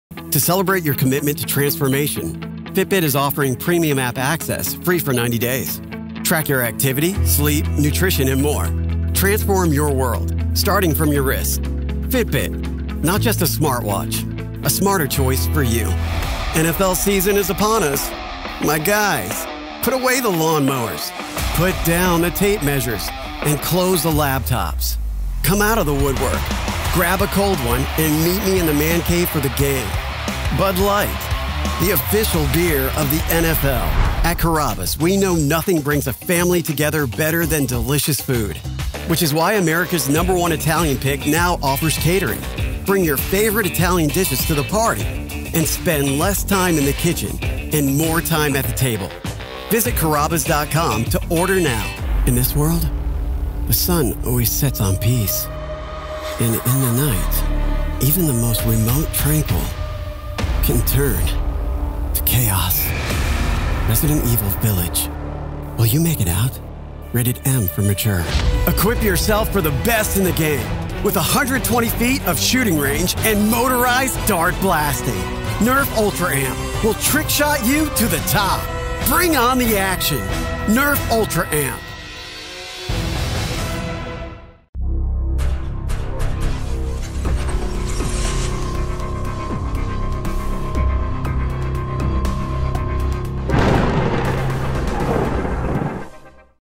Brad Storm | Commercial Demo Reel
I had the pleasure of working with Tony Wijs with Immersed Productions on my latest and greatest Commercial Demo Reel.

We discussed what my favorite things to do were and Tony provided the scripts that fit my personality to a T.  I'm passionate about exercise, the NFL, Italian food, video games and really enjoy providing my voice to children toy commercial spots.

If you are looking for an intense experience that will reap results, look no further than Tony Wijs and Immersed Productions!

They punch demos in the FACE!!